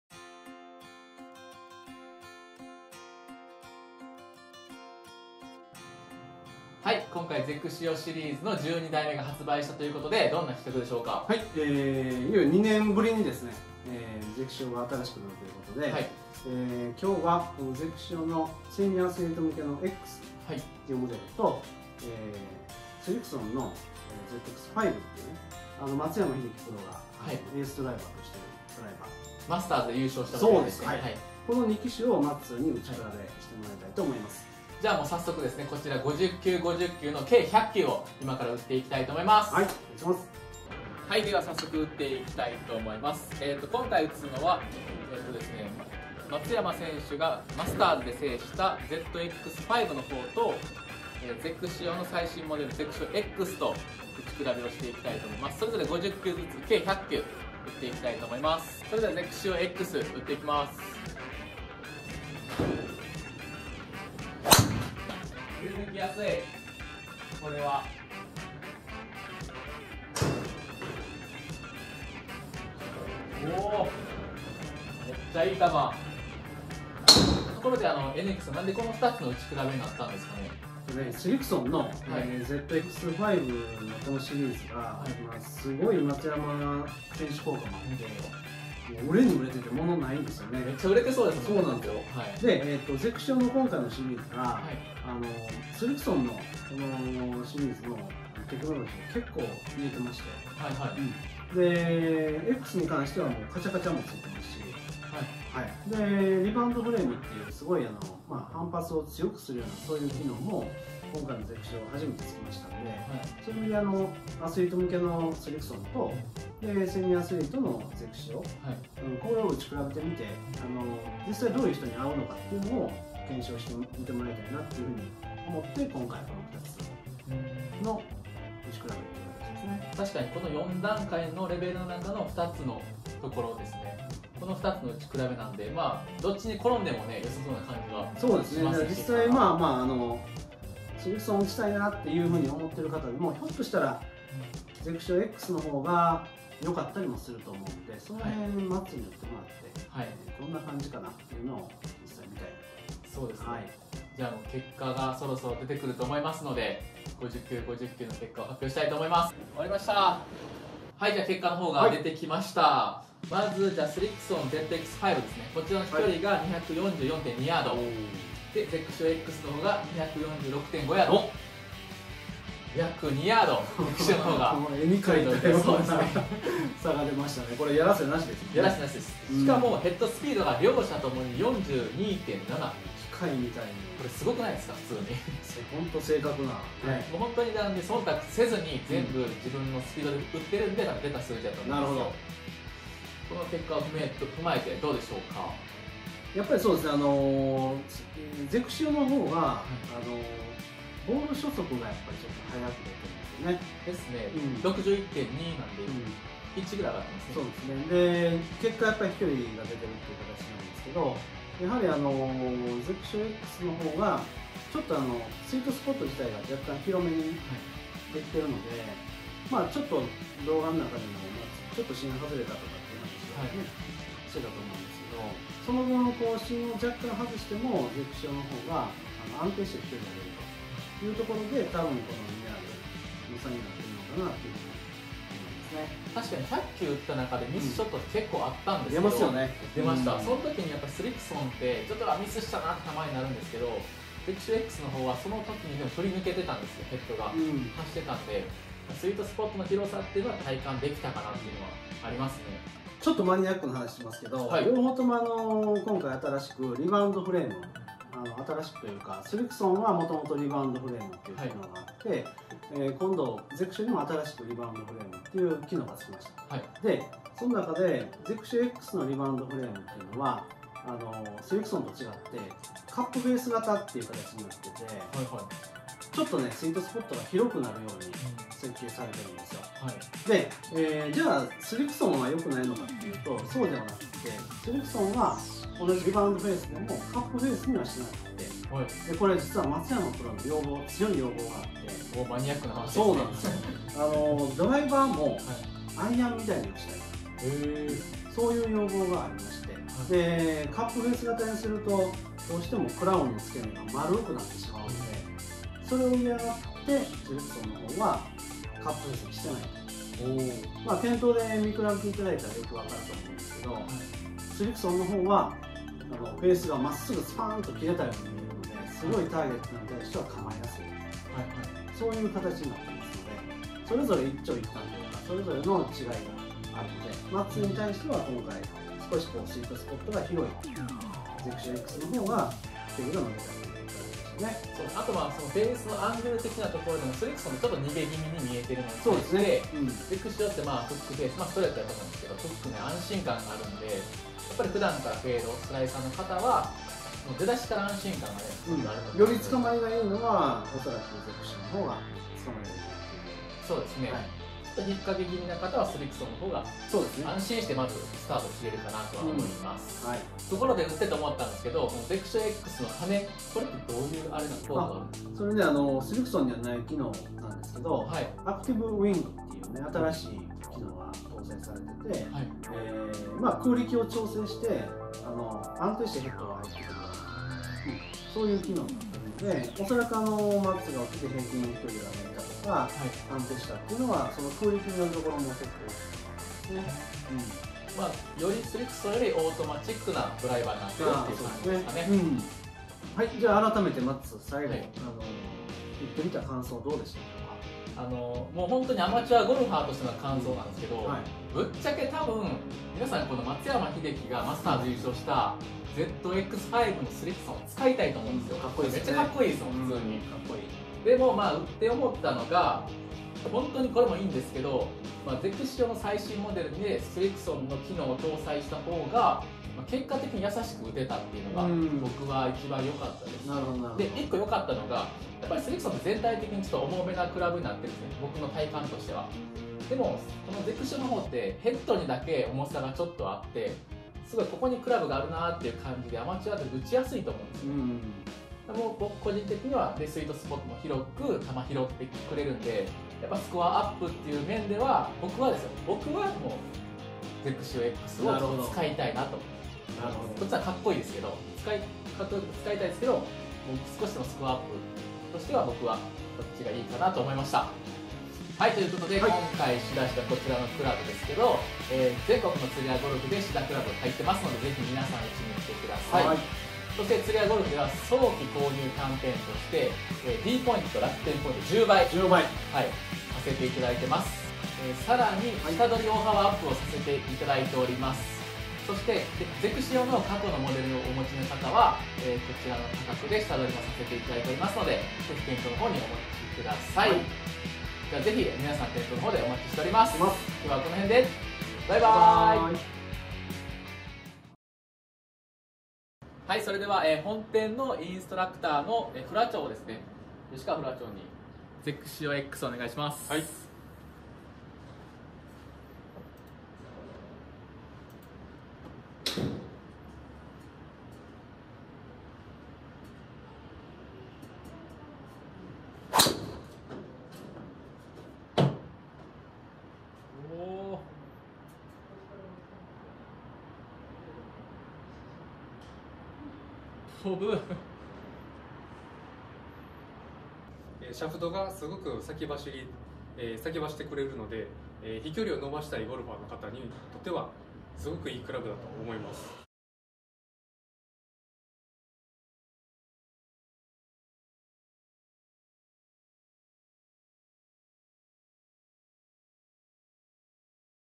松山選手愛用（SRIXON ZX-5）と最新モデル（XXIO X）100球試打レビュー
アベレージゴルファーの忖度なし！１００球レビュー！！！

１１月４日発表のダンロップ最新モデルXXIO_Xと松山英樹選手がマスターズを制した時に使用していたSRIXON　ZX_５を１００球打ち比べしてみました。

・１１月　５日（金曜）より全国のつるやゴルフにて試打クラブ入荷
・１２月１１日（土曜）XXIO X発売予定

つるやONLINEからでも予約可能です！

つるやゴルフ公式アプリ:
お得情報満載のアプリが誕生!

つるやゴルフSNS:
ゴルフのことなら # 教えてつるッピー で何でもお答えします!